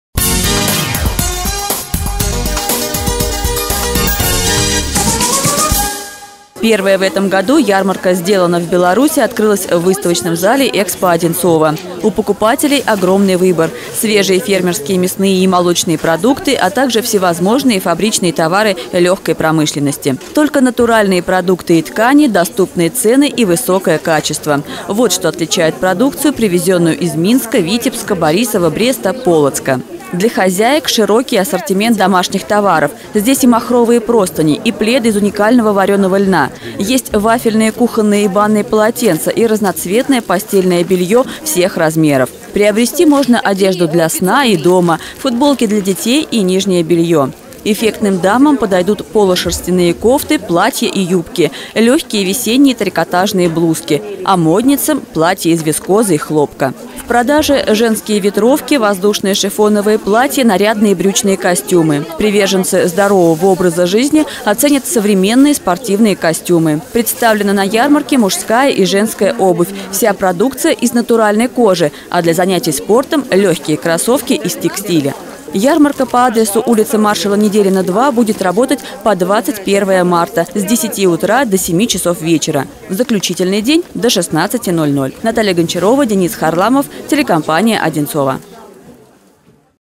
Сделано в Беларуси
Фермерские продукты, товары легкой промышленности и косметическая продукция представлены в выставочном зале «ЭКСПО-Одинцово» на ярмарке «Сделано в Беларуси». Она будет работать по 21 марта.